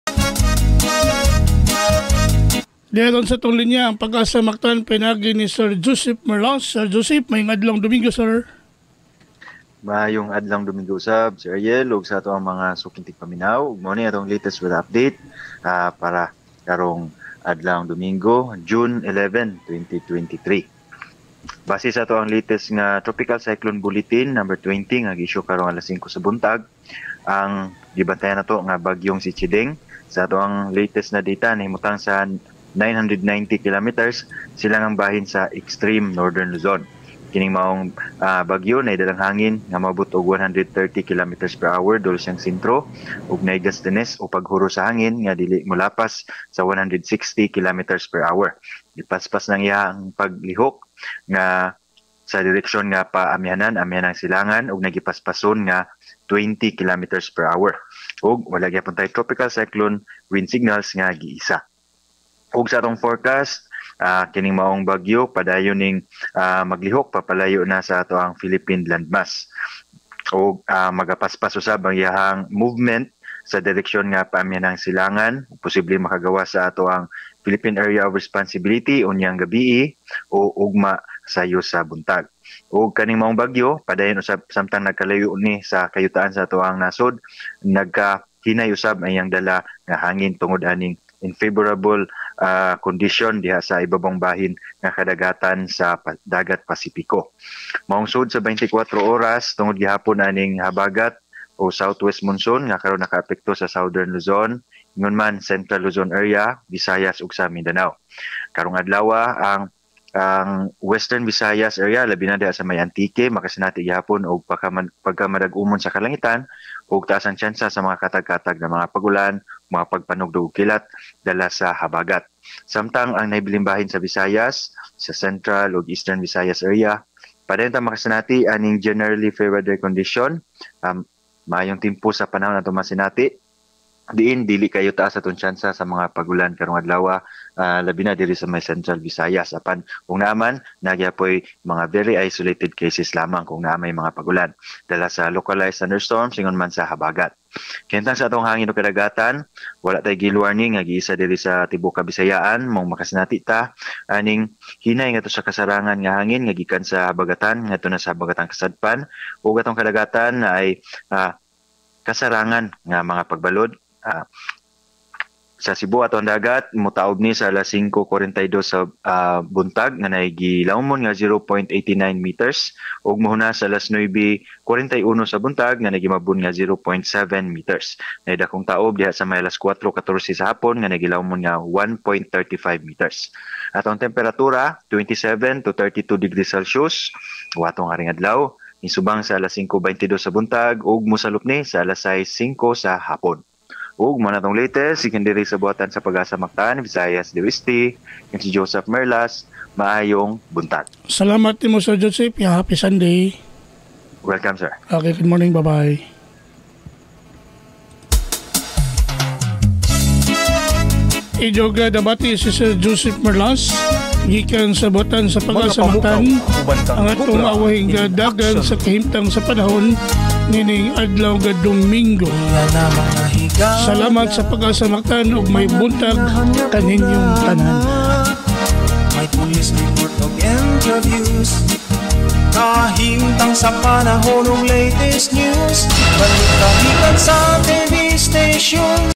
WEATHER UPDATE TINGOG NI Joseph Merlas Weather Specialist ,PAGASA Visayas
WEATHER UPDATE
TINGOG NI Joseph Merlas
Weather Specialist ,PAGASA Visayas

JUNE 11,2023 SUNDAY MGA PAMUKAW UBAN KANG COBRA IN AKSYON.

( This content is for Weather Update purposes